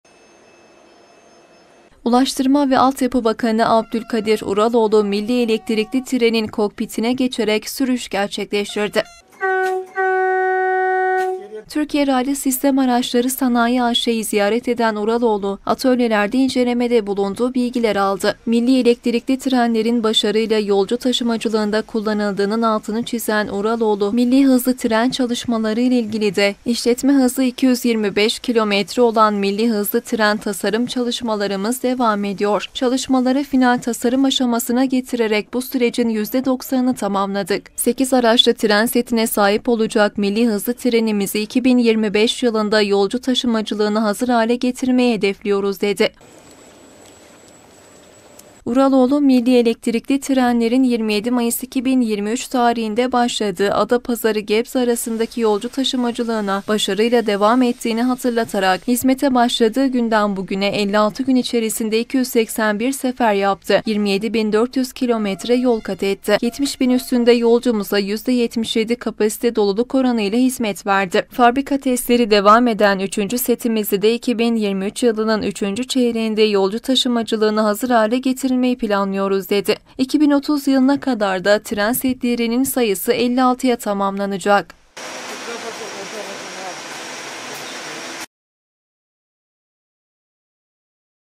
URALOĞLU MİLLİ ELEKTRİKLİ TRENİ KULLANDI
''Milli hızlı trenimizi 2025 yılında yolcu taşımacılığına hazır hale getireceğiz''